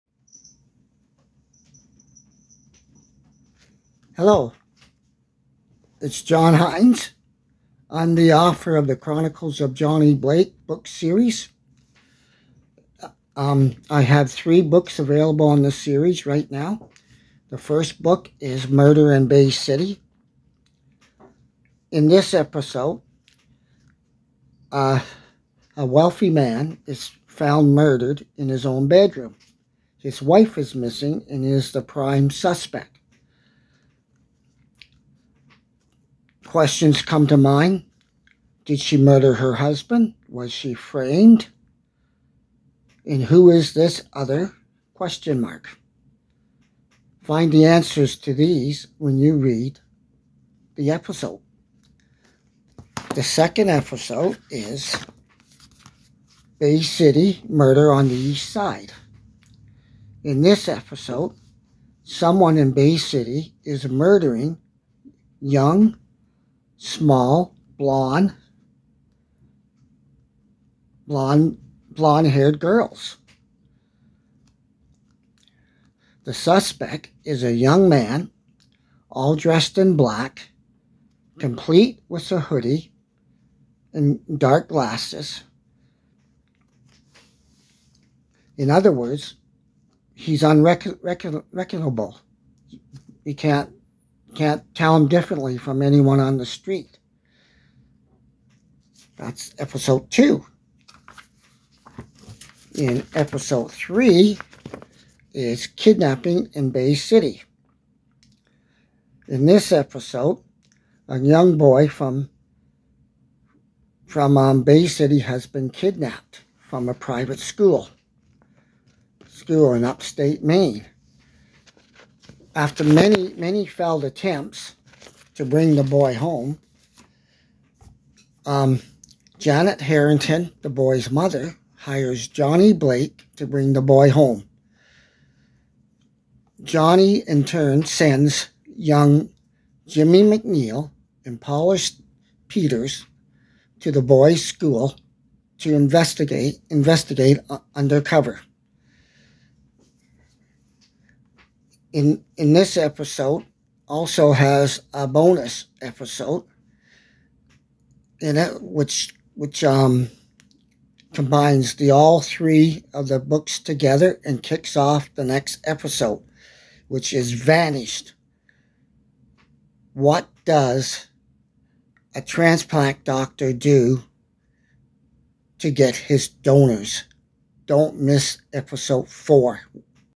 The Chronicles of Johnny Blake Book Series
This amazing series is a modern-day romantic Murder mystery and suspense thriller with an old-time feel.Written in the author styles of authors Mark Twain and Mickey Spillane's Mike Hammer Book and TV series. Trilogy one box set of the first three episodes. you get episodes Murder in Bay City. In this awesome first episode a wealthy man is found murdered in his bedroom. his wife is missing and is the prime suspect.
THE HEADLINE READ WHERE IS ELIZABETH HARRINGTON!
Did she murder her husband? Was she framed? Who is this other...? Find the answers to these questions and more when you read this awesome first episode.
Bay City Murder on the East Side.
ln this amazing 2nd episode, someone in Bay City is murdering small short hair blonde young girls. A suspect is a young man all dr
essed in black complete with a hoodie and dark glasses  other words unrecognizable.
Kidnapping in Bay City episode three.
In this episode a young boy from Bay Ci
 and find out what they undercover. What is the connection to episode one Murder in Bay City.  Thank you for reading, John Hynes author.